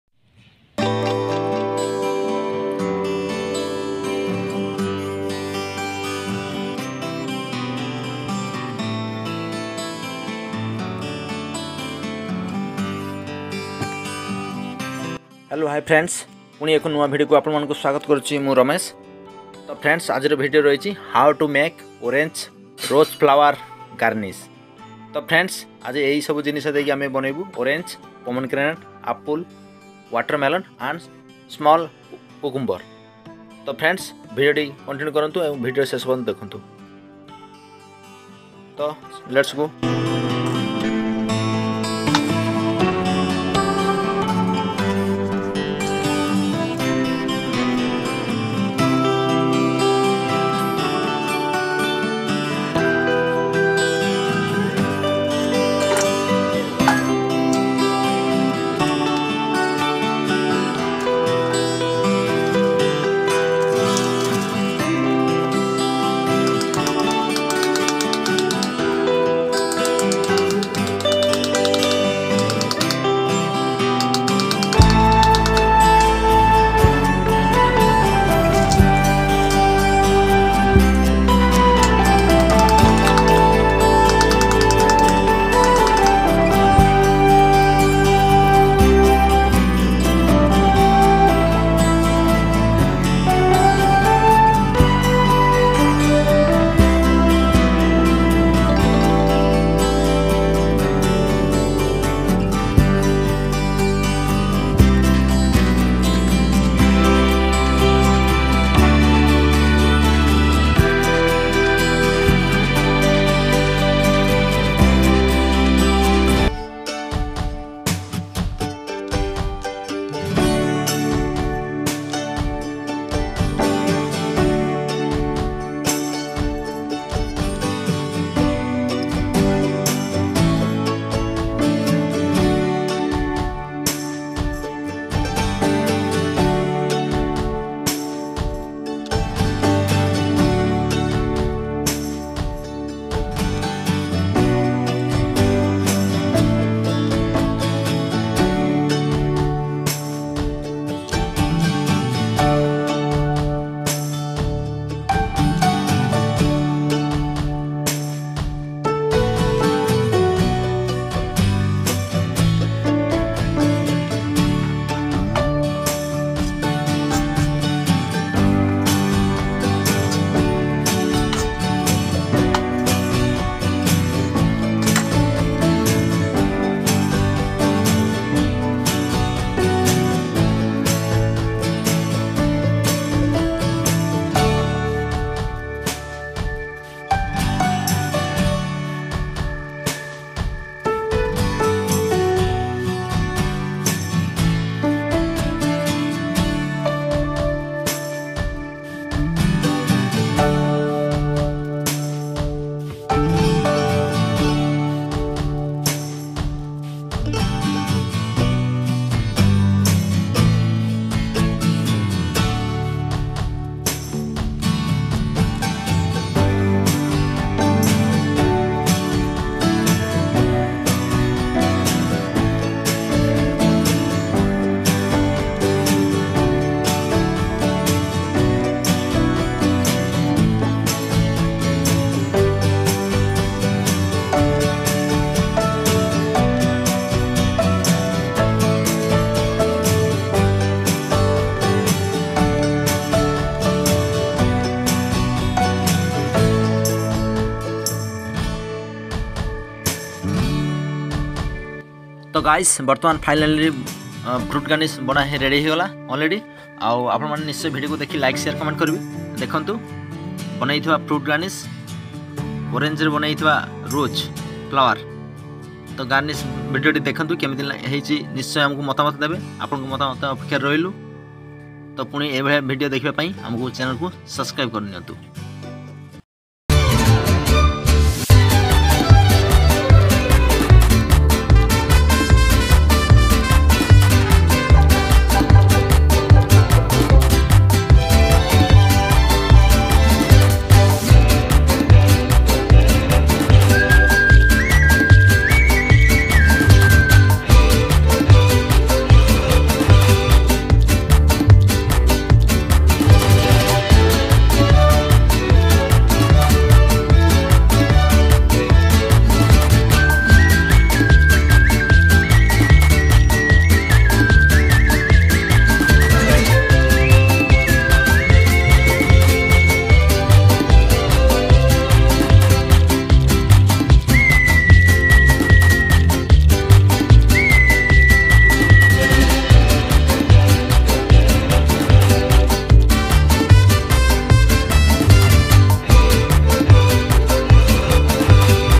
How to make orange Rose flower garnish ||🌹 କେମିତି ସହଜରେ କମଳା ରେ ଗୋଲାପ ଫୁଲ ବନାଇବେ
Creat by Ramesh Chandra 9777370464 work as sailor in catamaran (luxury cruise bhitarkanika)